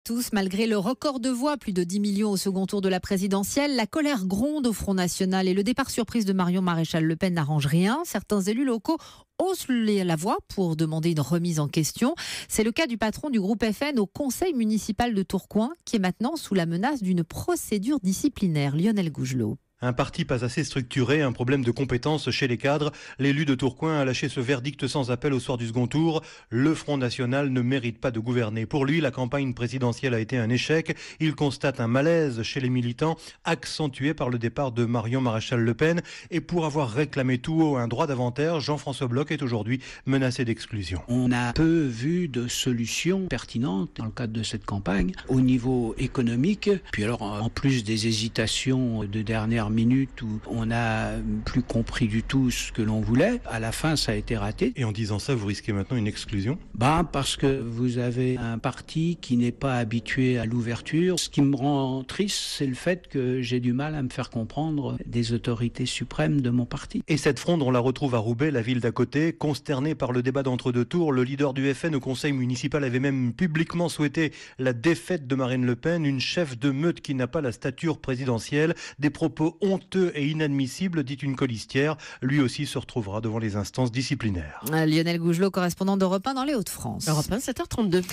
Crise au FN : procédure d'exclusion pour les déçus de Marine Le Pen
Alors que le parti frontiste doit encaisser le départ de Marion Maréchal-Le Pen, il doit aussi faire face à la fronde des déçus de la campagne.